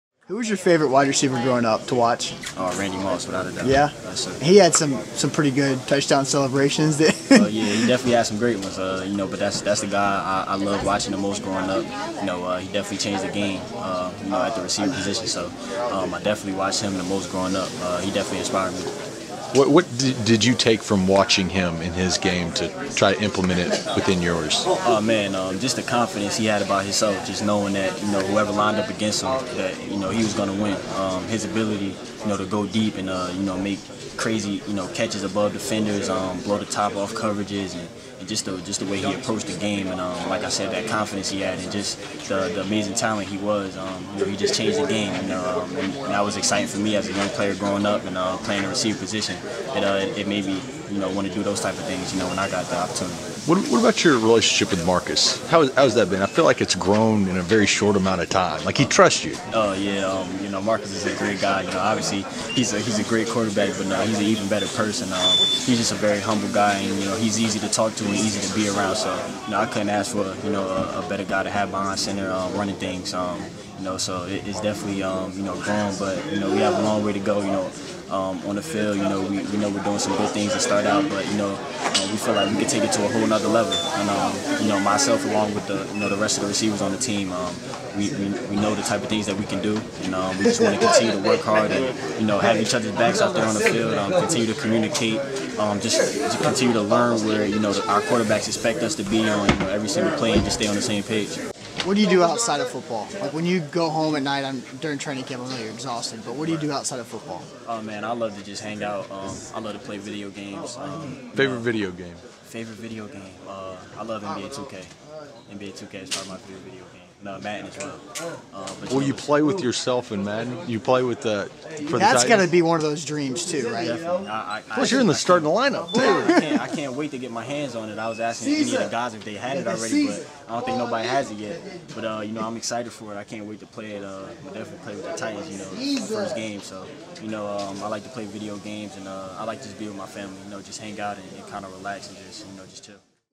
Tajae Sharpe on his WR idol, Marcus Mariota & video games
Titans rookie WR Tajae Sharpe tells us who he modeled his game after, touches on his relationship with QB Marcus Mariota & his favorite video games